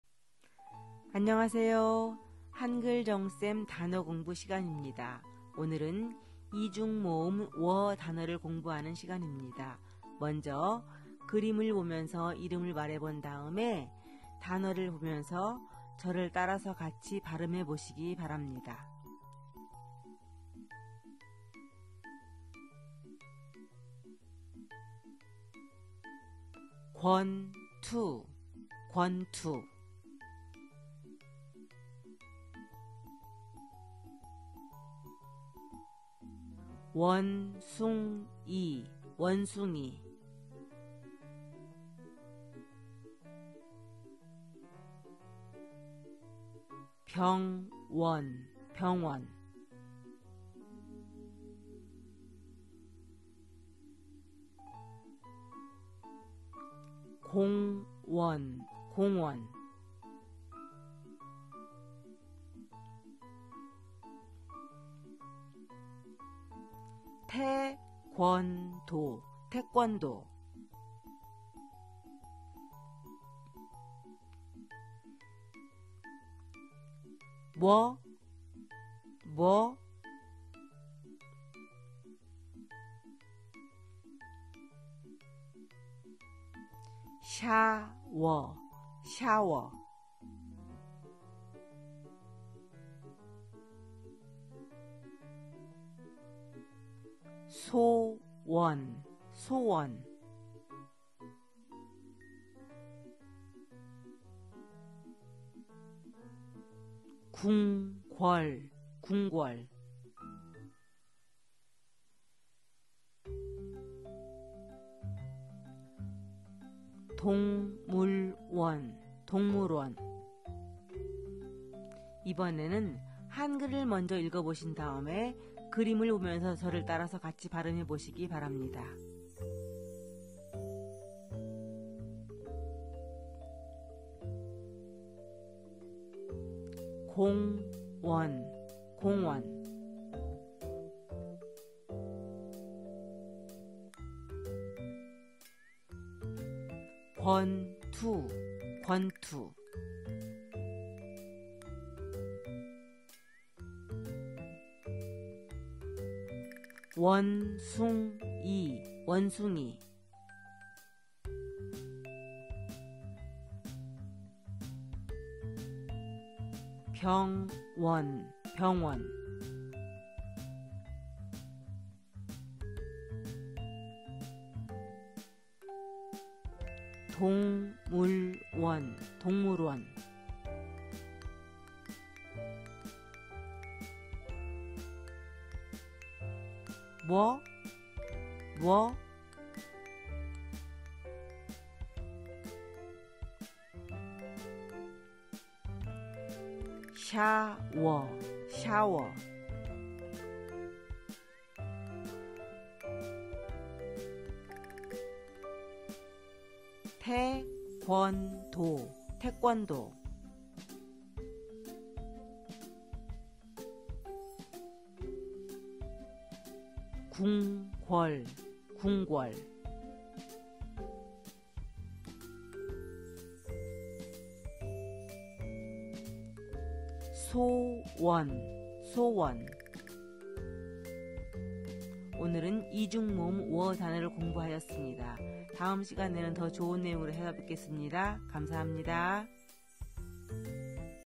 [한국어단어공부] 이중모음 ㅝ 단어 Korean Vocabulary
한글을 공부 할 때 외국인들이 어려워하는 발음이 겹받침과 이중모음입니다. 이중모음의 모양을 하고 있으면서도 단모음으로 발음하는 경우도 있고, 이중모음이 오는 순서에 따라서 발음이 달라지기도 해서 많은 학생들이 처음 한글을 배울 때 어려워합니다.  많이 쓰이는 단어들을 중심으로 많이 발음해 보고 글자를 눈에 익혀서 한글을 좀 더 쉽게 배울 수 있도록 하였습니다. 매일 10분씩 투자해서 한글 단어를 공부 해 보세요.